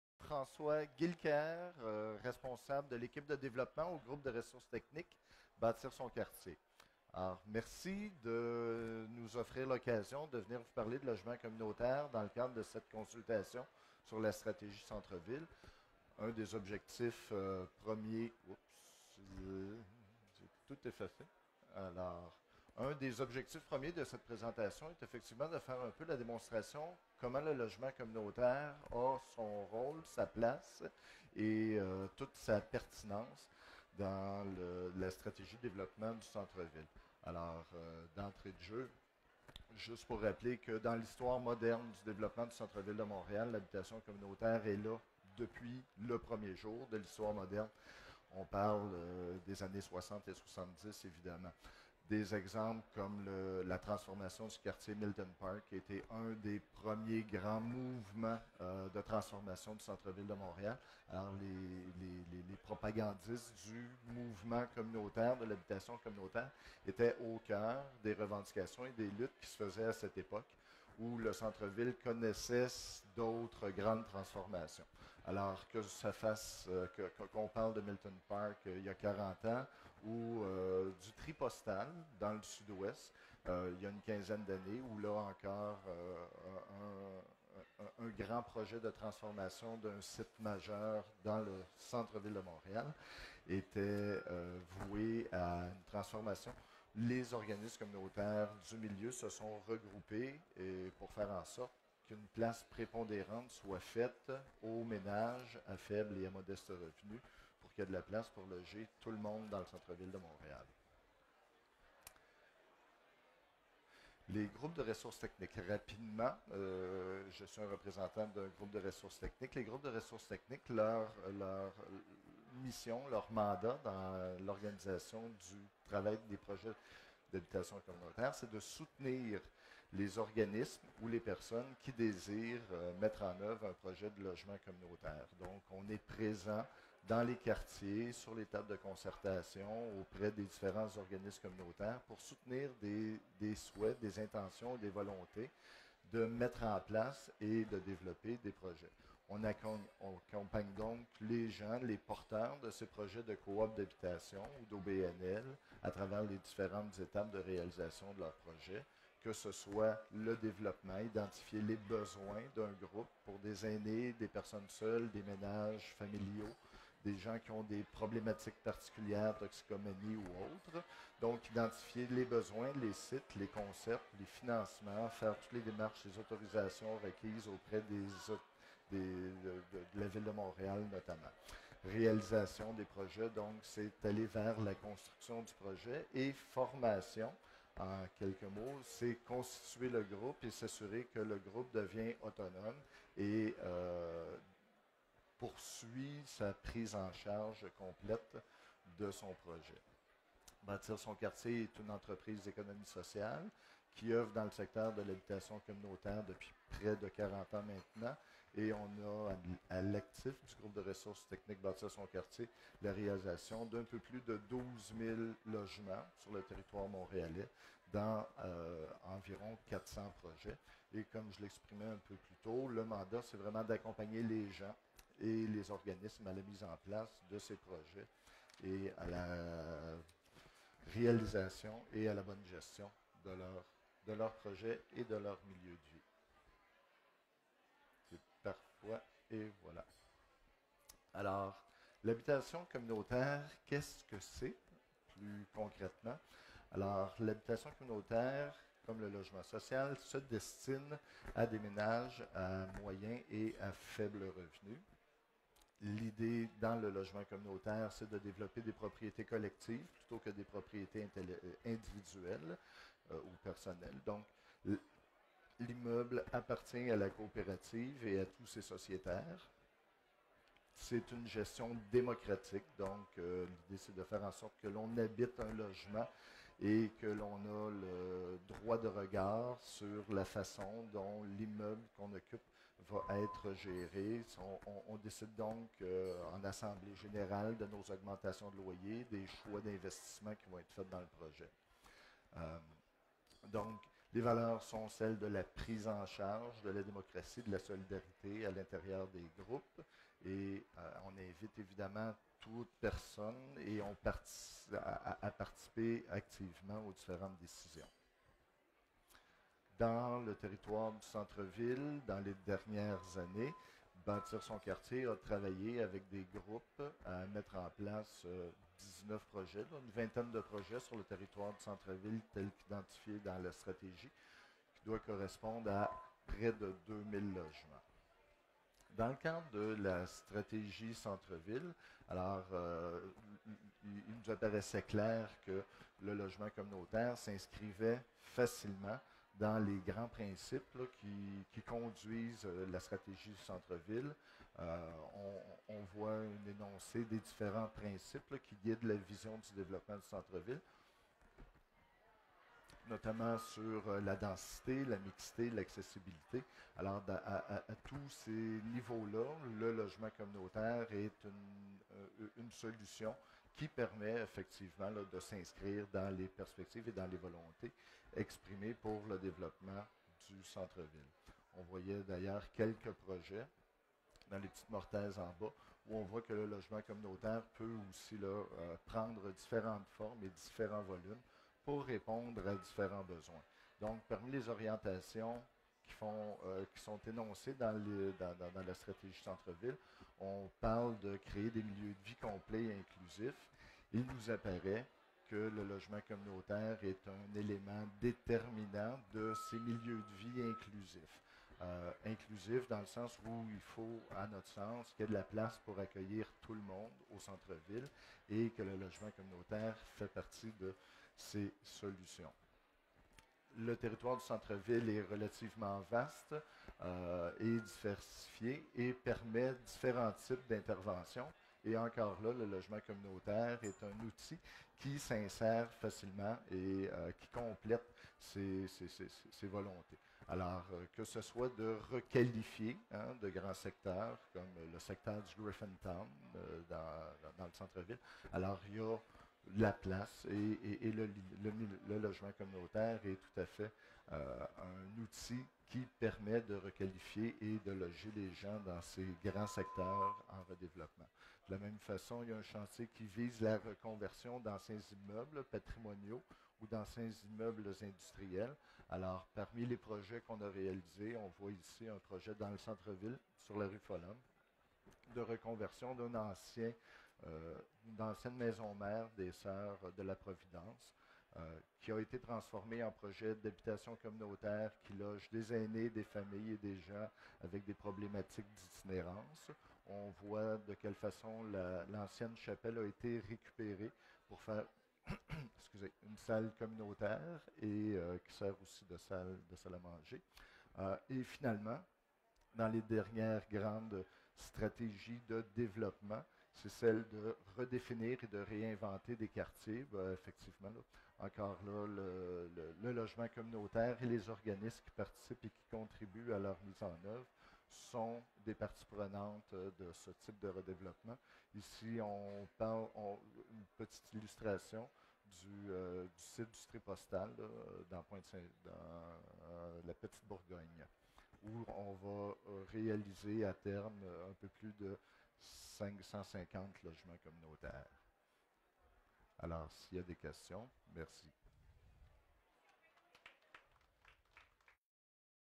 Centre-ville - Nanoconférence Bâtir son quartier
Nanoconférence de Bâtir son quartier lors du Grand rendez-vous du centre-ville du 25 septembre 2016, dans le cadre de la consultation publique de l'Office de consultation publique de Montréal sur la Stratégie centre-ville.